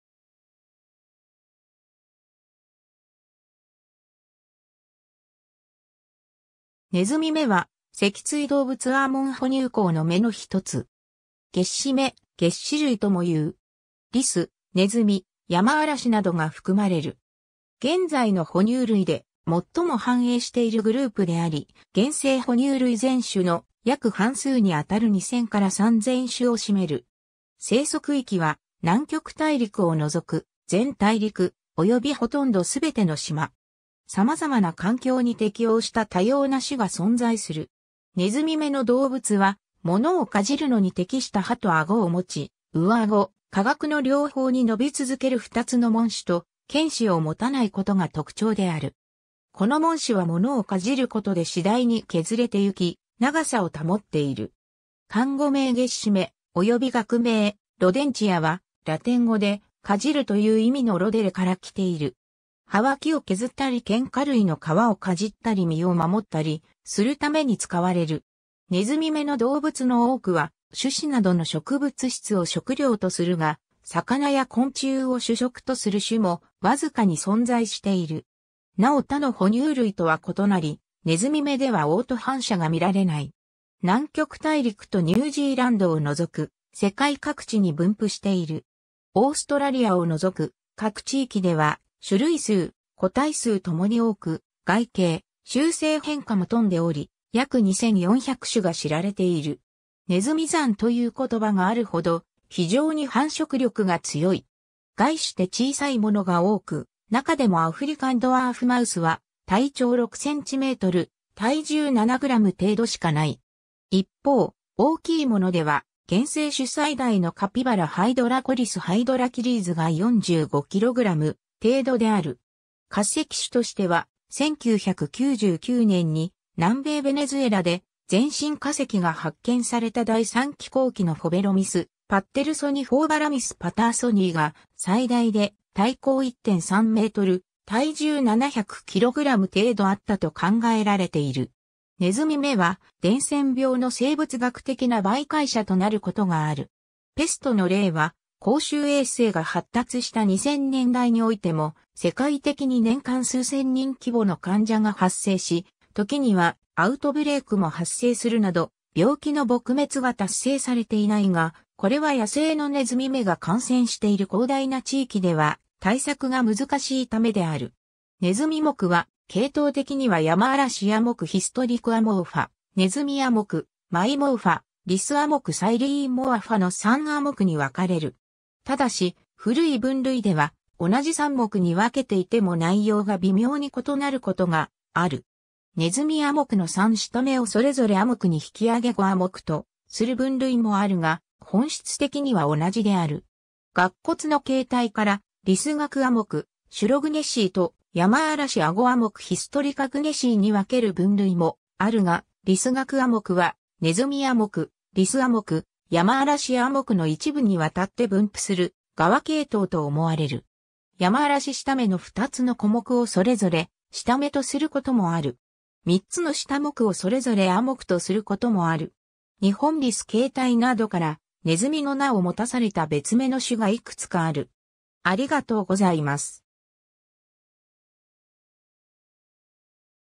ネズミ目
ネズミ目 (ネズミもく、Rodentia) は脊椎動物亜門 哺乳綱の目の1つ。
齧歯目（げっしもく）、齧歯類（げっしるい）ともいう。
リス、ネズミ、ヤマアラシなどが含まれる。
現在の哺乳類で最も繁栄しているグループであり、現生哺乳類全種（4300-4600種）の約半数に当たる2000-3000種を占める。
生息域は、南極大陸を除く全大陸、およびほとんどすべての島。
さまざまな環境に適応した多様な種が存在する。
 ネズミ目の動物は、物をかじるのに適した歯と顎を持ち、上顎・下顎の両方に伸び続ける2つの門歯と、犬歯を持たないことが特徴である。
この門歯は物をかじることで次第に削れてゆき、長さを保っている。
漢語名齧歯目、および学名「Rodentia」はラテン語で「かじる（齧る）」という意味のrodereから来ている。
歯は木を削ったり堅果類の皮をかじったり身を守ったりするために使われる。
ネズミ目の動物の多くは、種子などの植物質を食料とするが、魚や昆虫を主食とする種もわずかに存在している。
なお他の哺乳類とは異なり、ネズミ目では嘔吐反射が見られない。
 南極大陸とニュージーランドを除く世界各地に分布している。
オーストラリアを除く各地域では、種類数・個体数ともに多く、外形・習性変化も富んでおり、約2400種が知られている。
「ねずみ算」という言葉があるほど、非常に繁殖力が強い。
 概して小さいものが多く、なかでもアフリカンドワーフマウスは体長6cm、体重7g程度しかない。
一方、大きいものでは、現生種最大のカピバラ Hydrochoeris hydrochaeris が 45kg 程度である。
 化石種としては、1999年に南米ベネズエラで全身化石が発見された第三紀後期のフォベロミス・パッテルソニ Phoberomys pattersoni が最大で、体高 1.3 m（尾まで含めた体長は3 m）、体重 700 kg程度あったと考えられている。
 ネズミ目は、伝染病の生物学的な媒介者となることがある。
ペストの例は、公衆衛生が発達した2000年代においても世界的に年間数千人規模の患者が発生し、時にはアウトブレイクも発生するなど病気の撲滅が達成されていないが、これは野生のネズミ目が感染している広大な地域では対策が難しいためである。
 ネズミ目は系統的にはヤマアラシ亜目 Hystricomorpha (Ctenohystrica)、ネズミ亜目 Myomorpha、リス亜目 Sciuromorpha (Sciurognathi) の3亜目に分かれる。
ただし、古い分類では、同じ3目に分けていても内容が微妙に異なることがある。
 ネズミ亜目の3下目をそれぞれ亜目に引き上げ5亜目とする分類もあるが、本質的には同じである。
 顎骨の形態からリス顎亜目（リス亜目） Sciurognathi とヤマアラシ顎亜目（ヤマアラシ亜目） Hystricognathi に分ける分類もあるが、リス顎亜目はネズミ亜目・リス亜目・ヤマアラシ亜目の一部（グンディ科）にわたって分布する側系統と思われる。
 ヤマアラシ下目の2つの小目をそれぞれ下目とすることもある。
 3つの下目をそれぞれ亜目とすることもある。
 ニホンリス 形態などから、ネズミの名を持たされた別目の種がいくつかある。